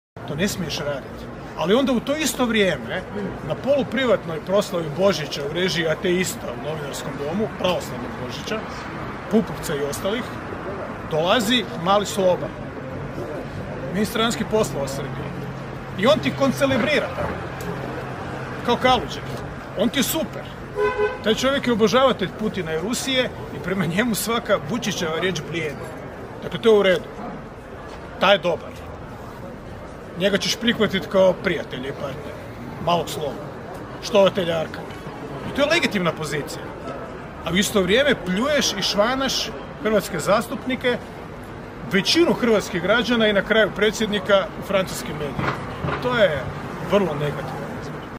Milanović o Plenkoviću: Mali Slobo je štovatelj Arkana i on ti je super
Obilježavanje 31. godišnjice međunarodnog priznanja Republike Hrvatske i 25. obljetnice završetka mirne reintegracije hrvatskog Podunavlja te završetka mandata UNTAES-a u Hrvatskom domu. Predsjednik RH Zoran Milanović dao je izjavu medijima.